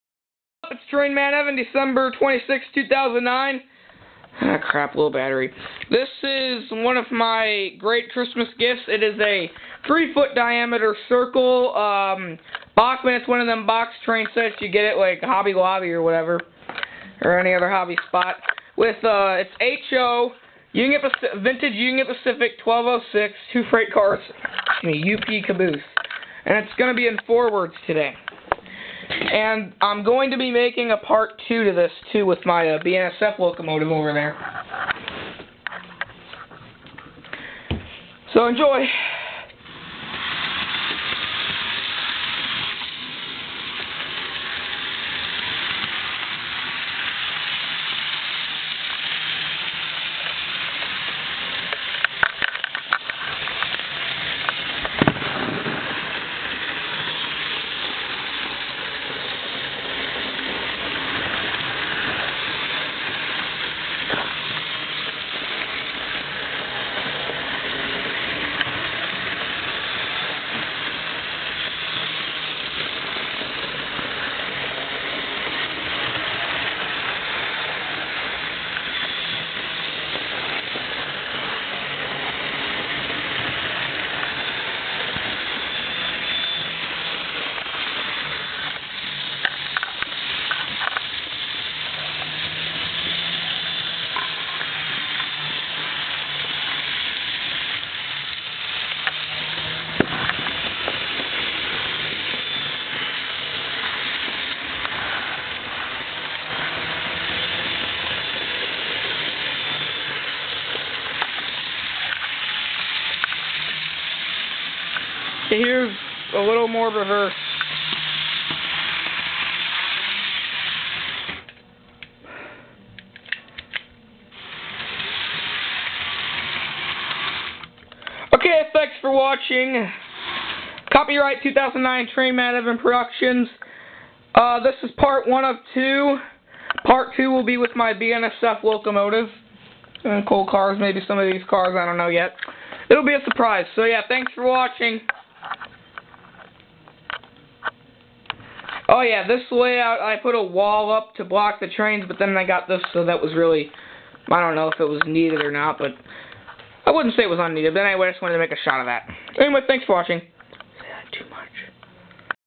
12/26/09- Just messin' with my Bachman train set I got for Christmas- Part 1/2
PLEASE RATE AND COMMENT!!! This is one of my very good Christmas gifts. It's a 3 Foot diamater circle set made by Bachman with a Union Pacific EMD F9 Diesel #1206 and 2 frieght cars with a UP caboose. I will be expanding it, so don't think it's gonna be like this forever! I also got a Walthers BNSF diesel #3820 and 2 coal cars. I also got a New York Central steam locomotive, but I haven't ran it yet. :(
BTW, I'll be making a part 2 with my BNSF soon. :)